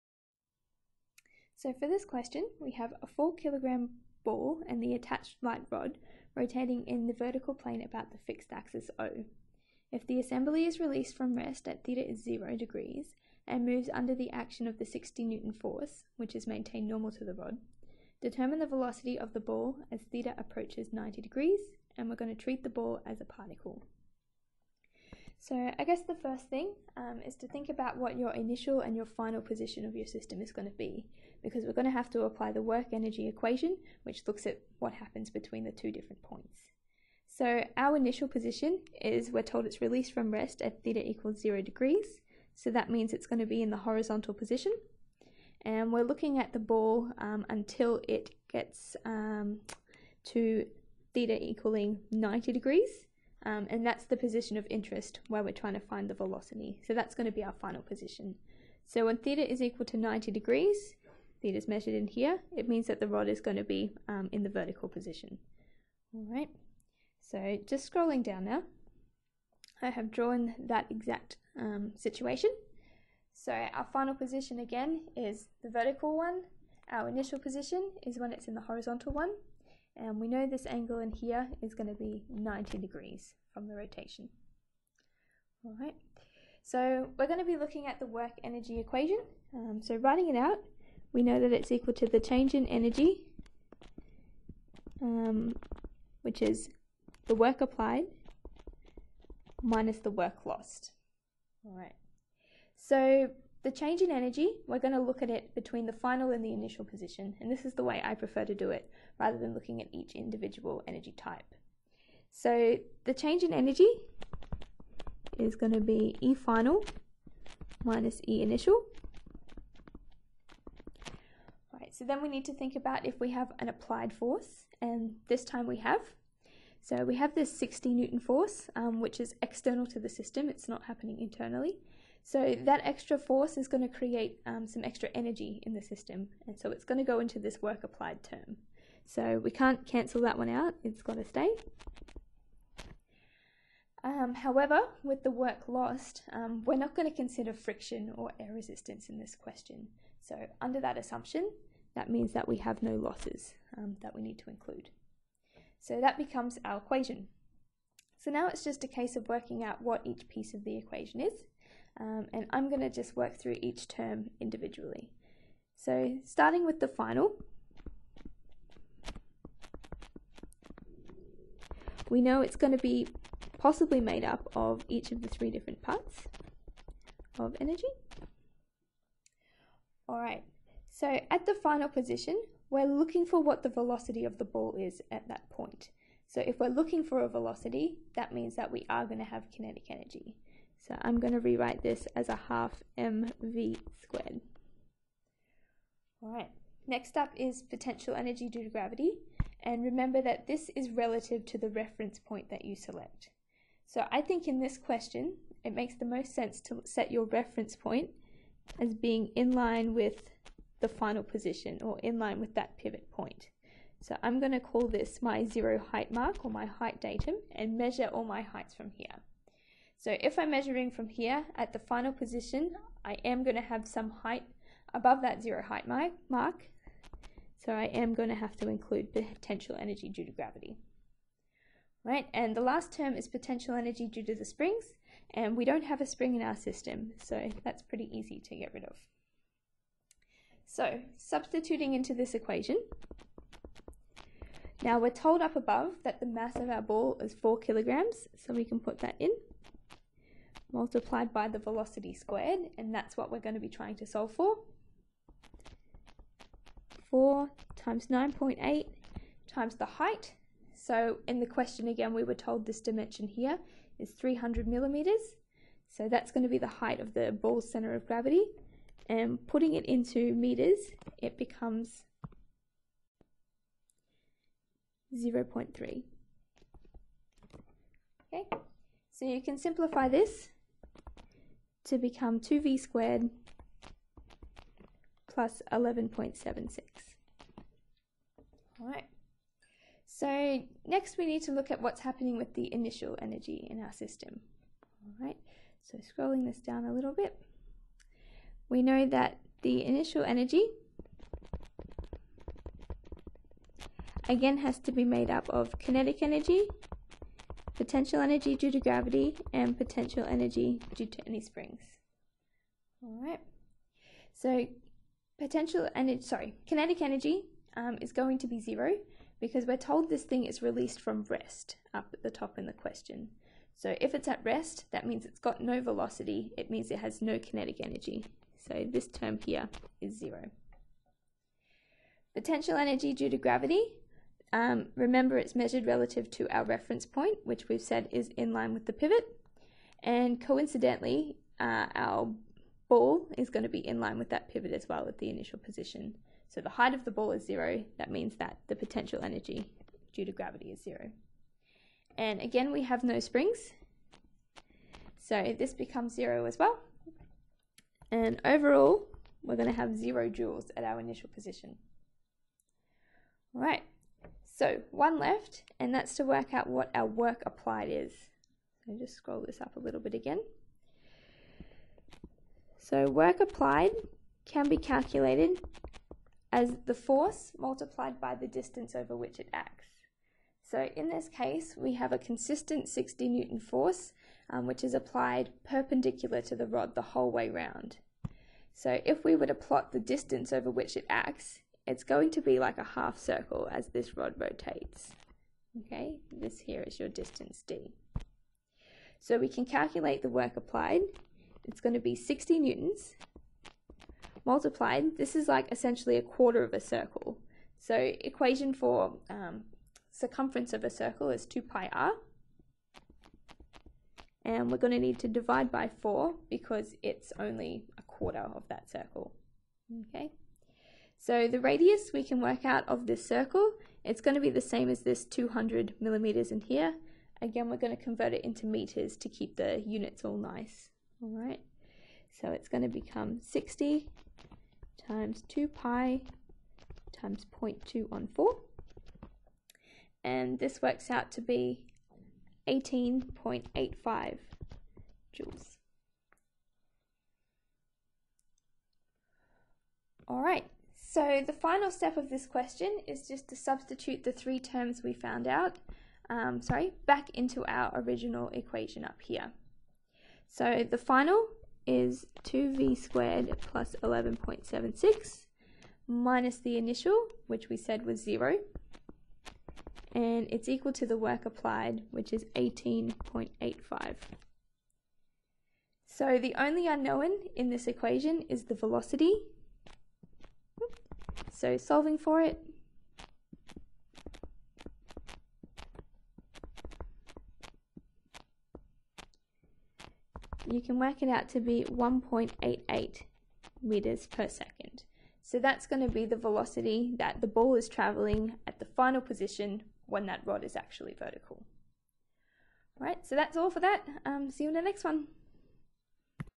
(3/112) Rotating Ball with Applied Work
This video finds the velocity of a ball which is rotating about a pivot when a force is applied. This is done using an energy method.